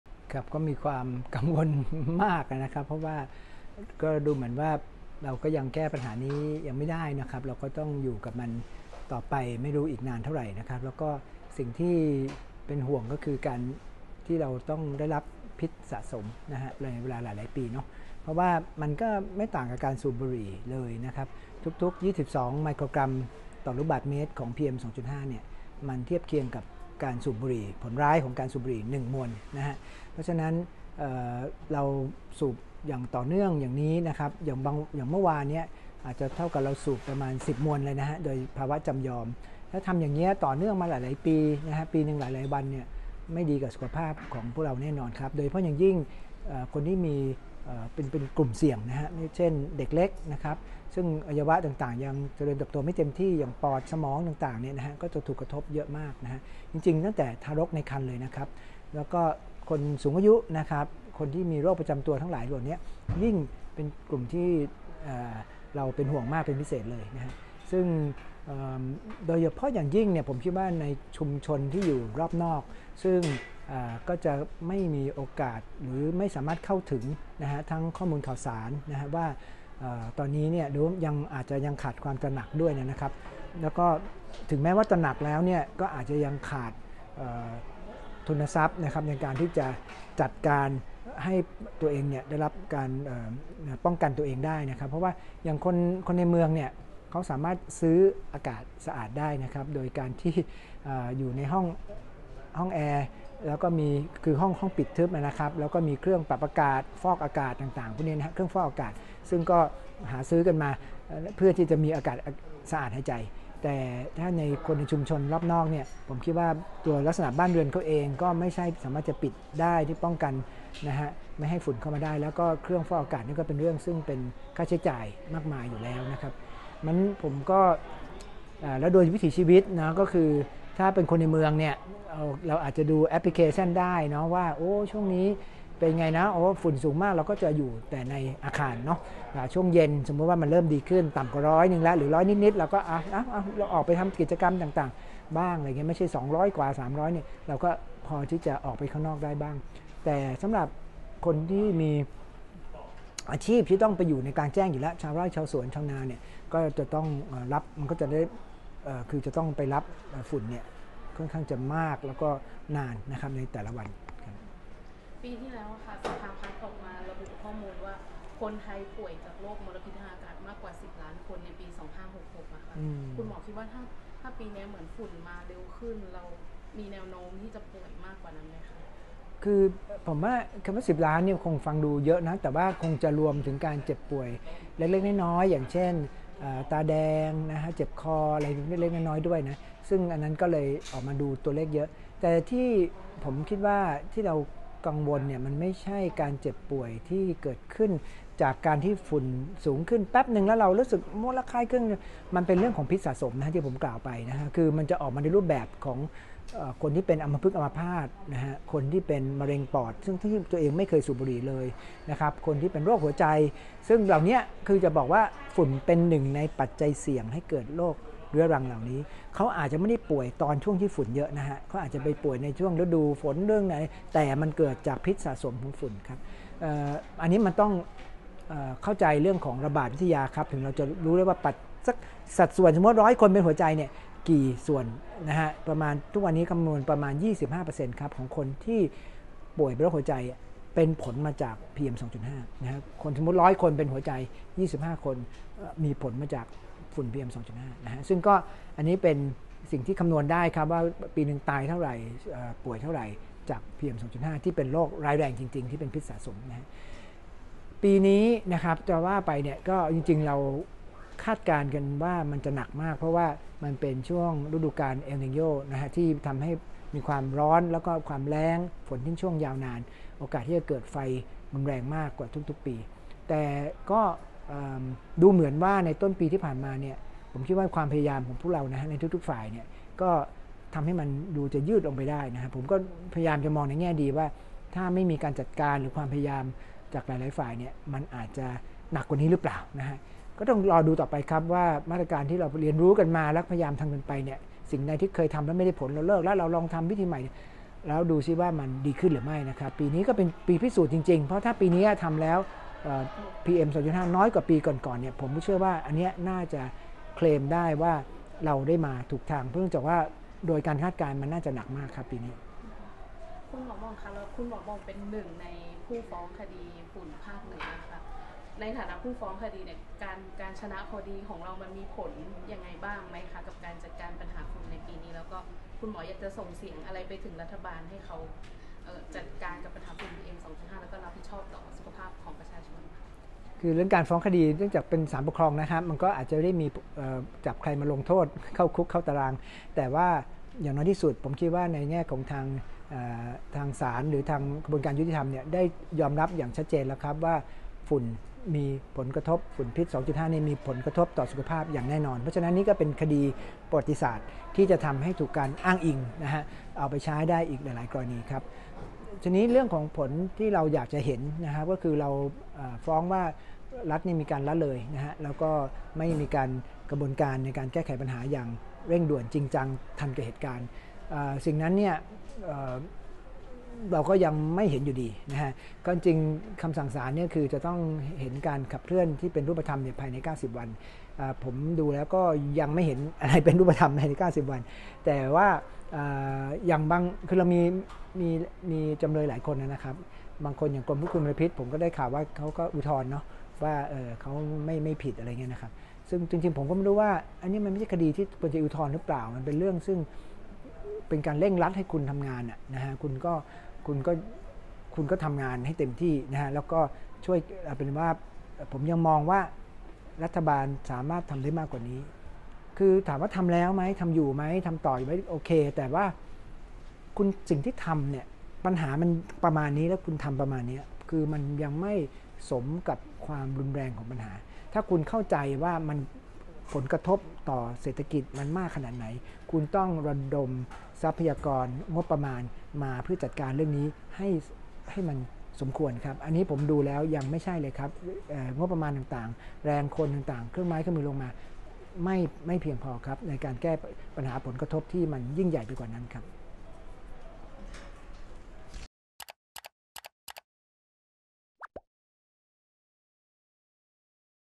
'นพ.รังสฤษฎ์' ห่วงคนเหนือสูดฝุ่น PM2.5 สะสมจนป่วย
8 มี.ค. 67 ประชาไทสัมภาษณ์ นพ.รังสฤษฎ์ กาญจนะวณิชย์ หรือ 'หมอหม่อง' แพทย์ผู้เชี่ยวชาญด้านโรคหัวใจ โรงพยาบาลมหาราชนครเชียงใหม่ เกี่ยวกับปัญหา ฝุ่นพิษ ภาคเหนือซึ่งกำลังเป็นที่น่ากังวล เนื่องจากส่งผลกระทบระยะยาวต่อสุขภาพของประชาชนในพื้นที่ โดย นพ.รังสฤษ ชี้ว่าฝุ่นเป็นหนึ่งในปัจจัยเสี่ยงที่ทำให้เกิดโรคต่าง ๆ เช่นกรณีของผู้ป่วยโรคหัวใจที่อาจคาดการณ์สัดส่วนว่าเกิดจากการสูดฝุ่นพิษต่อเนื่องได้ถึงประมาณร้อยละ 25